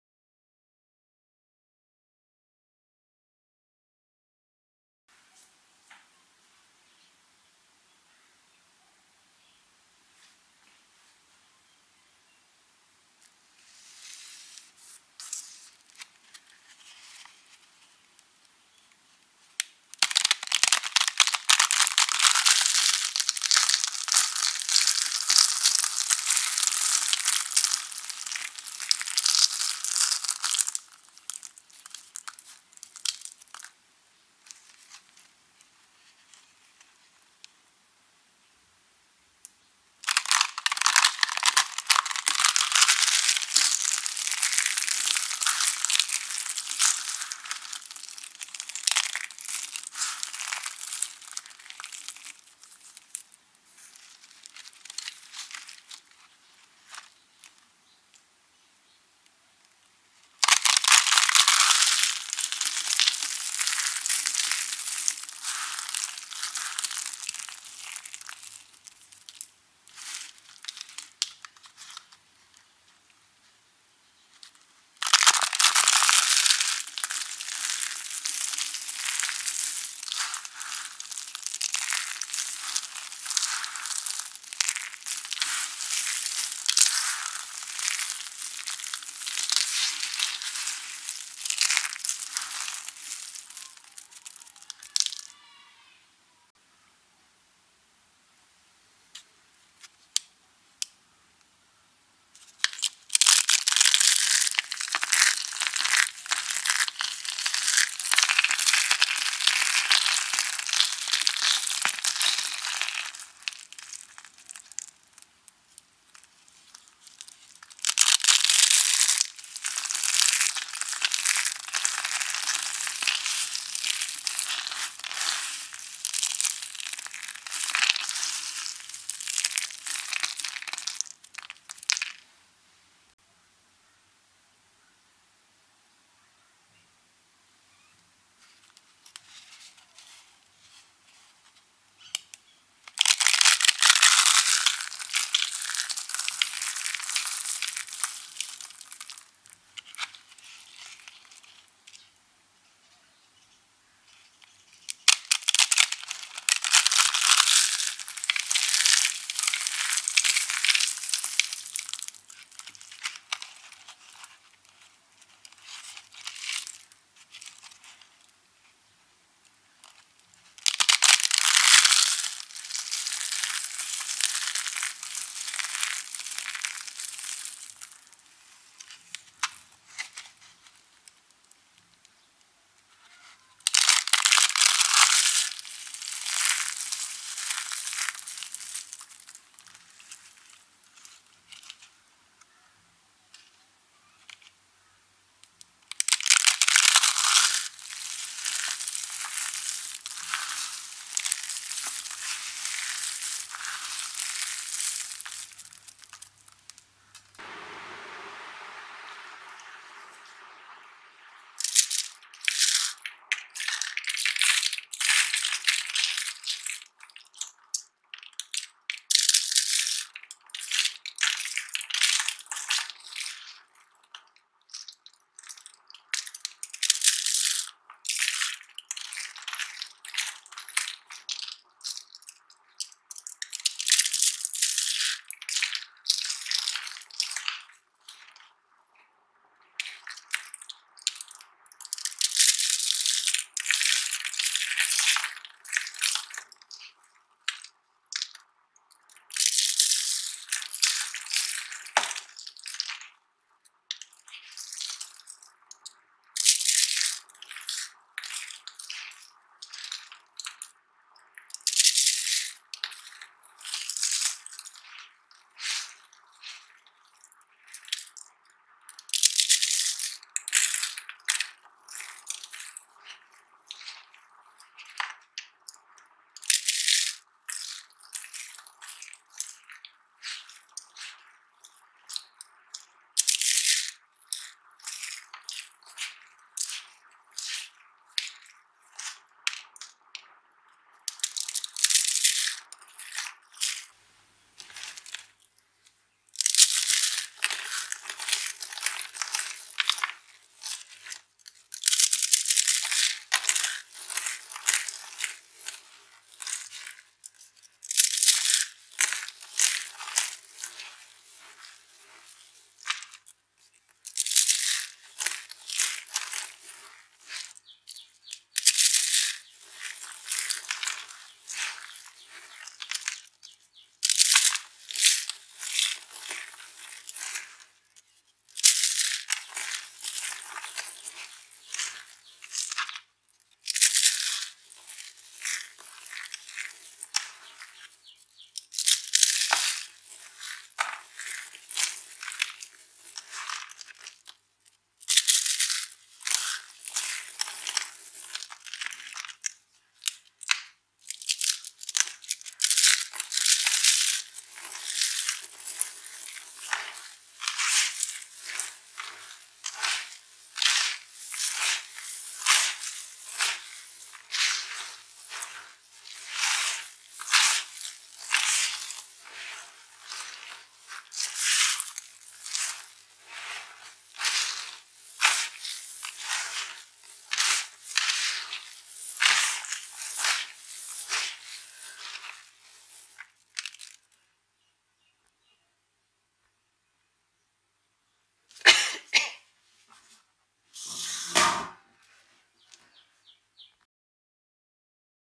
Satisfying Soap ASMR ! Dry Soap Curls/Roses
Satisfying ASMR Dry Crunchy Soap Sounds ! Relaxing Soap Crushing ASMR
Hello there !
Hope you enjoy this multi color crunchy  soap Crushing  :)

Don't use  my content.